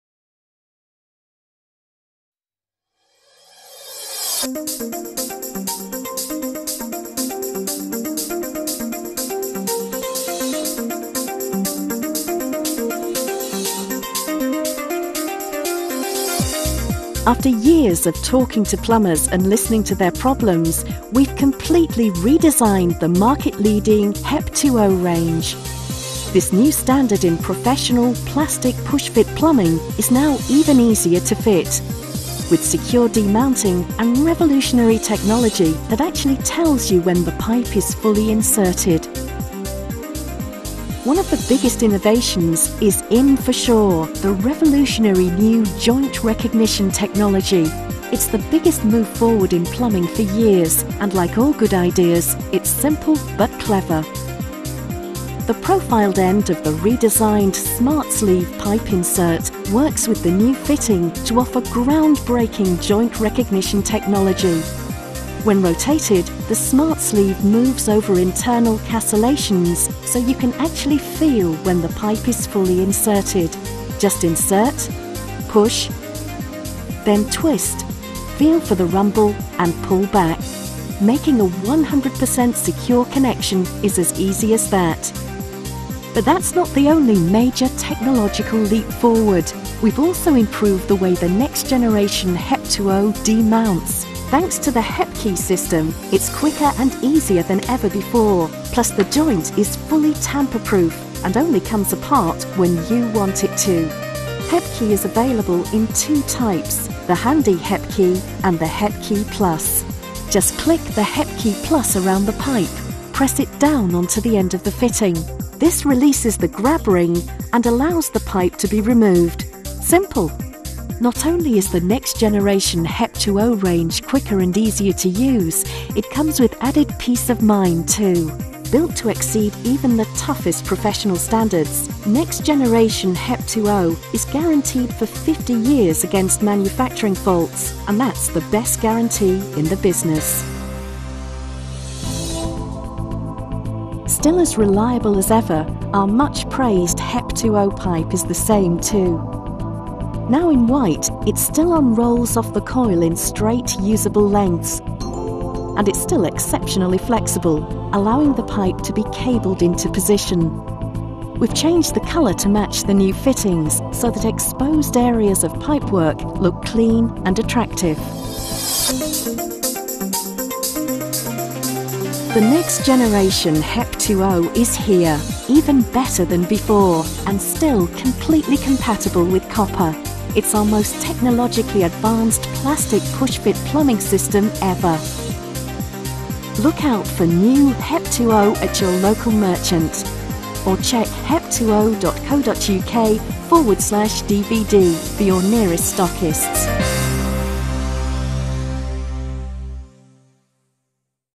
Hep2O - a promotional video with visualisation animation by WAK Studios
A promotional video produced for client Hep2O Film footage by Richard Measures at Real World Tv.
3D Animation created by WAK Studios, using Autodesk Maya.